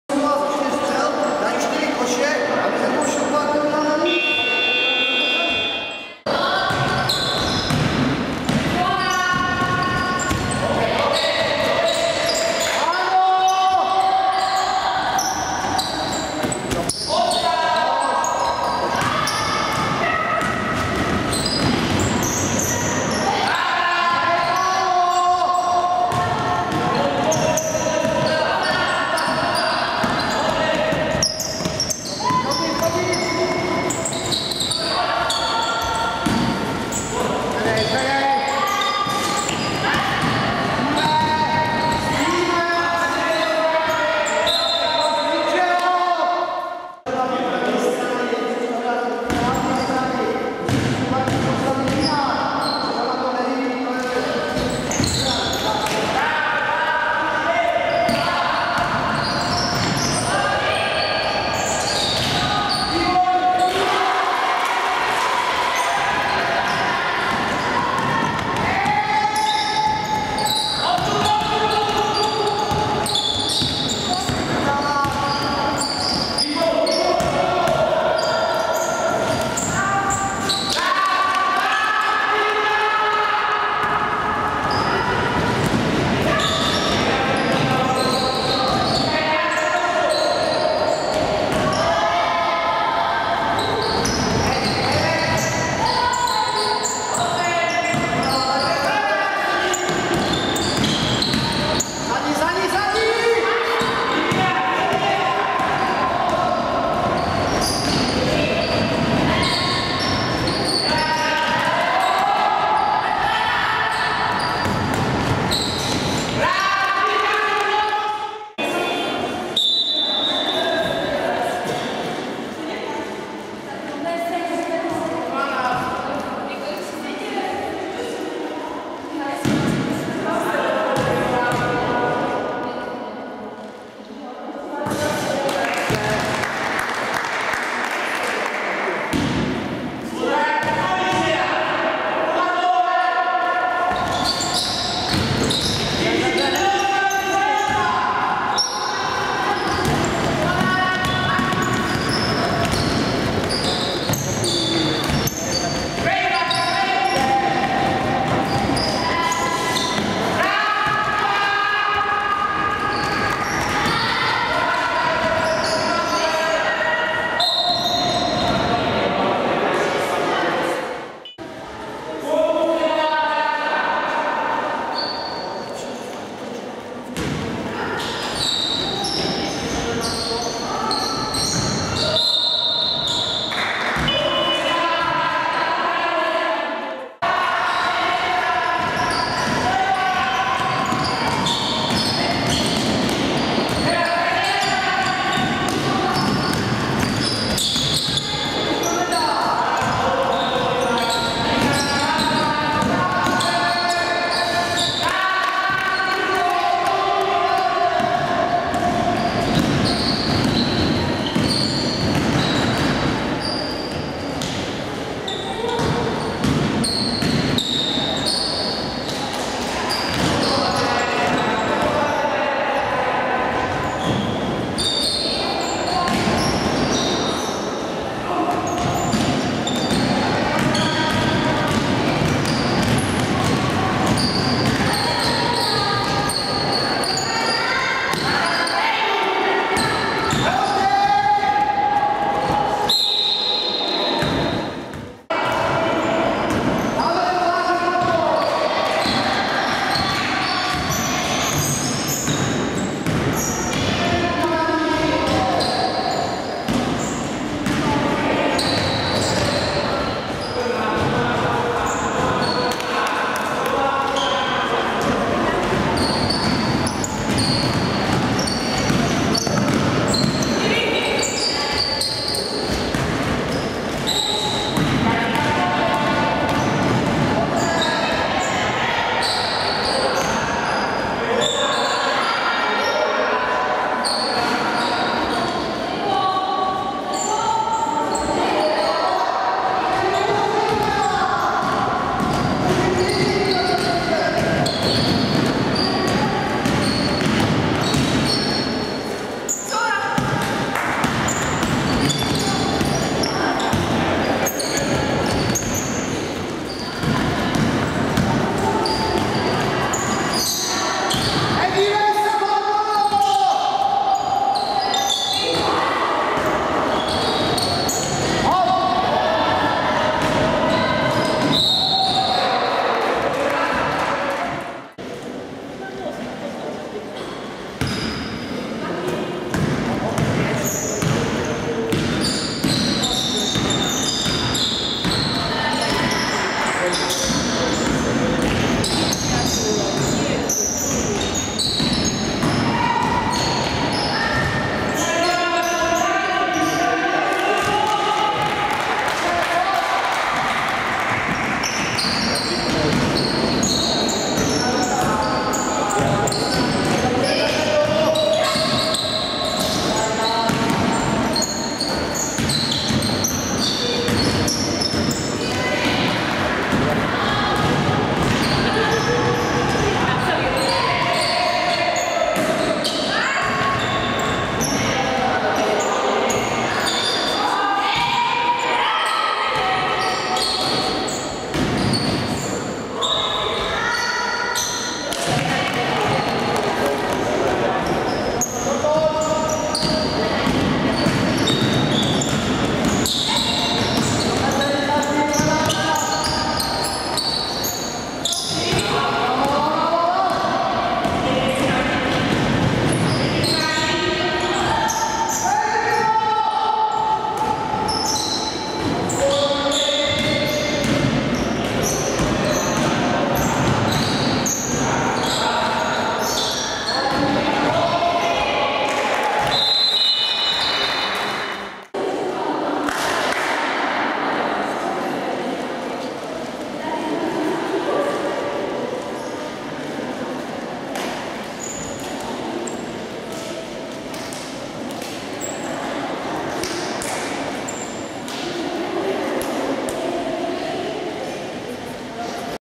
OSK Olomouc - BK SOUŽ Cassovia Košice 81:46 (st. žiačky)
Trojkráľový basketbalový turnaj vo Zvolen 2014